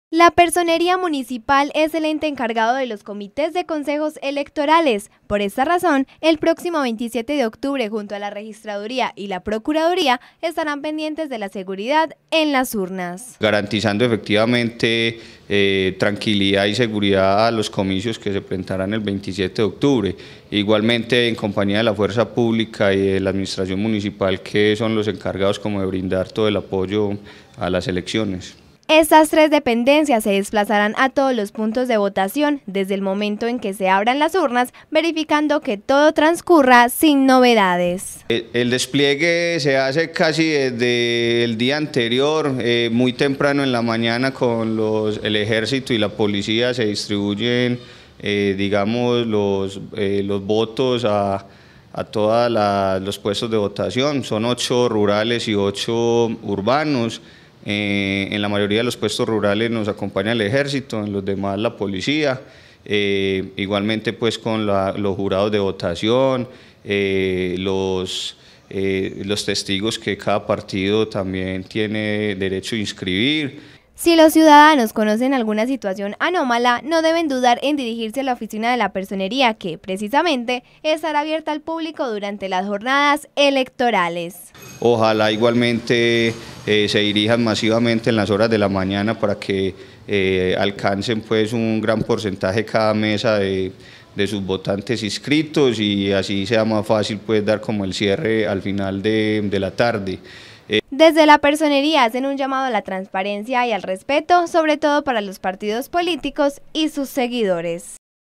RECOMENDACIONES DE LA PERSONERÍA PARA ESTE 27 DE OCTUBRE
Se acerca el 27 de octubre, fecha en la que se eligen los dirigentes regionales, y las dependencias encargadas de velar por la seguridad de este día, ya tienen sus estrategias para que los ciudadanos puedan ejercer su derecho al voto con tranquilidad.